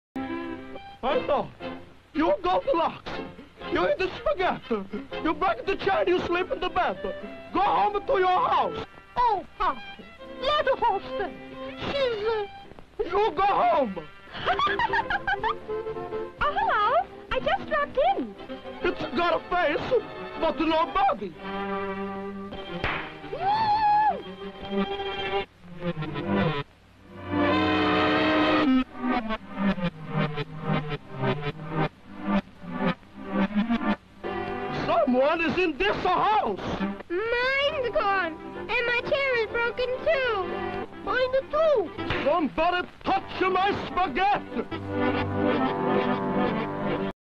SOMEBODY TOUCHA MY SPAGHET but it's reversed but what they say is still going forward
bad meme

I'm out of ideas